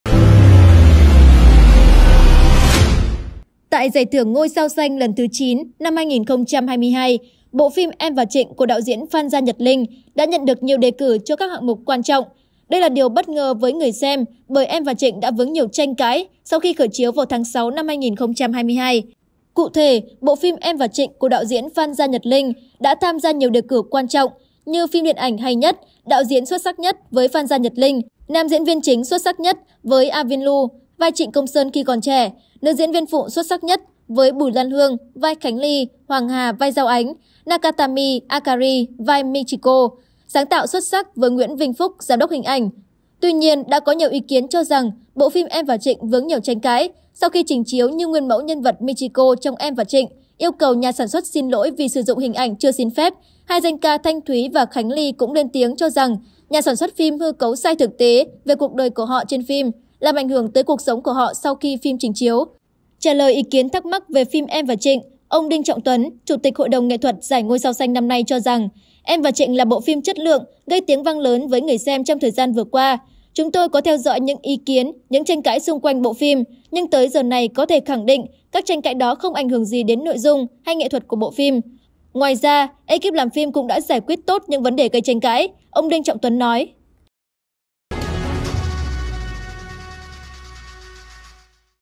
Vướng tranh cãi, 'Em và Trịnh' vẫn được đề cử giải thưởng
Tại Giải thưởng Ngôi sao Xanh lần thứ 9 năm 2022, bộ phim Em và Trịnh của đạo diễn Phan Gia Nhật Linh đã nhận được nhiều đề cử cho các hạng mục quan trọng. Đây là điều bất ngờ với người xem bởi Em và Trịnh đã vướng nhiều tranh cãi sau khi khởi chiếu vào tháng 6/2022.

Cụ thể, bộ phim Em và Trịnh (Đạo diễn Phan Gia Nhật Linh) đã tham gia nhiều đề cử quan trọng như Phim điện ảnh hay nhất; Đạo diễn xuất sắc nhất (Với Phan Gia Nhật Linh); Nam diễn viên chính xuất sắc nhất (Với Avin Lu - vai Trịnh Công Sơn khi còn trẻ); Nữ diễn viên phụ xuất sắc nhất (Với Bùi Lan Hương - vai Khánh Ly, Hoàng Hà - Vai Dao Ánh, Nakatani Akari - vai Michiko); Sáng tạo xuất sắc (Với Nguyễn Vinh Phúc - Giám đốc hình ảnh).

Tuy nhiên, đã có nhiều ý kiến cho rằng bộ phim Em và Trịnh vướng nhiều tranh cãi sau khi trình chiếu như nguyên mẫu nhân vật Michiko trong Em và Trịnh yêu cầu nhà sản xuất xin lỗi vì sử dụng hình ảnh chưa xin phép, 2 danh ca Thanh Thuý và Khánh Ly cũng lên tiếng cho rằng nhà sản xuất phim hư cấu sai thực tế về cuộc đời của họ trên phim, làm ảnh hưởng tới cuộc sống của họ sau khi phim trình chiếu…

Trả lời ý kiến thắc mắc về phim Em và Trịnh, ông Đinh Trọng Tuấn - Chủ tịch HĐNT giải Ngôi sao Xanh năm nay cho rằng, Em và Trịnh là bộ phim chất lượng, gây tiếng vang với người xem trong thời gian vừa qua. “Chúng tôi có theo dõi những ý kiến, những tranh cãi xung quanh bộ phim nhưng tới giờ này có thể khẳng định các tranh cãi đó không ảnh hưởng gì đến nội dung hay nghệ thuật của bộ phim. Ngoài ra, ê-kíp làm phim cũng đã giải quyết tốt những vấn đề gây tranh cãi" - Ông Đinh Trọng Tuấn nói.